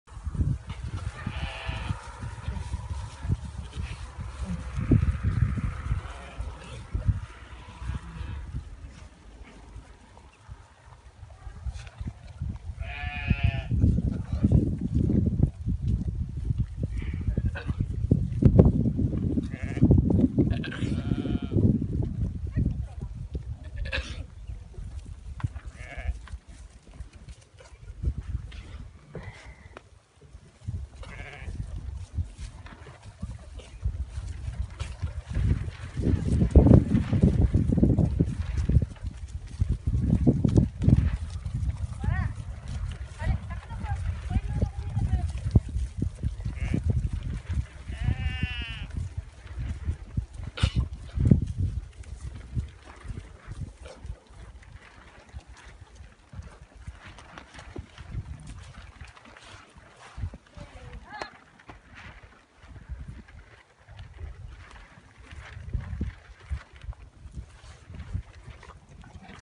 My Merino Sheep
German Merino Sheep. My roughage consumption is very good. I can supply this beautiful breed to those who want to have an affordable price.